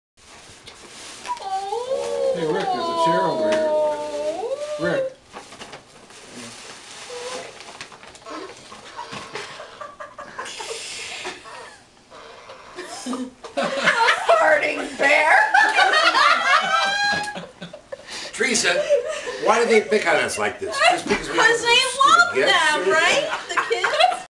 My aunt and the farting teddy bear!
Christmas is always interesting with my family, and this year was no exception.  here's the gift my aunt got from all of us!